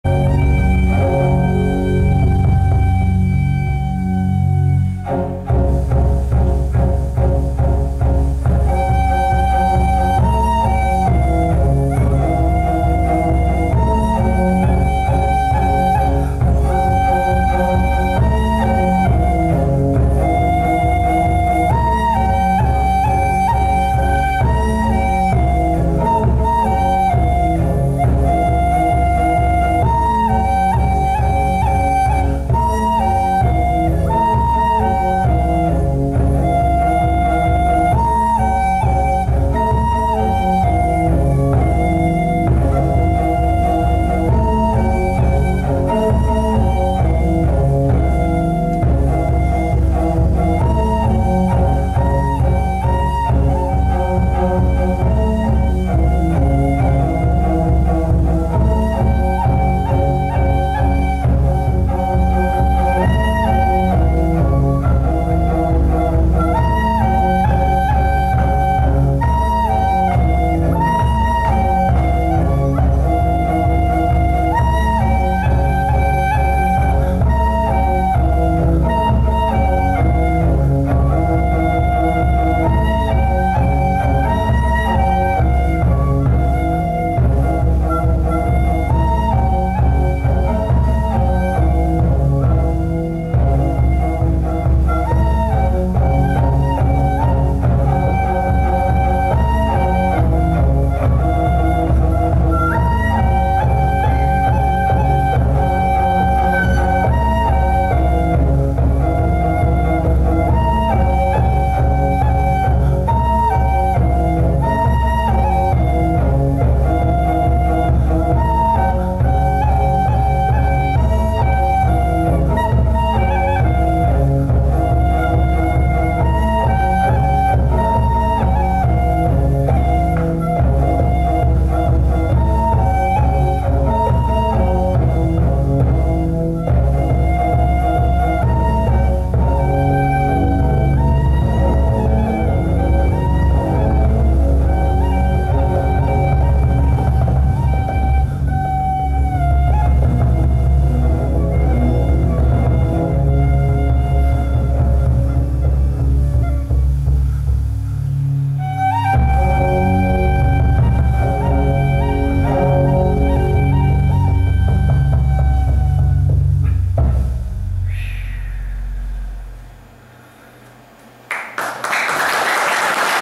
Concierto "MONTAÑAS SAGRADAS" - INKAFEST 2013
Tiro la Rosa - Tavo Castillo(grupo Fragil) - Pepe Chiriboga en el concierto de inauguración del INKAFEST mountain film festival 2013 - Huaraz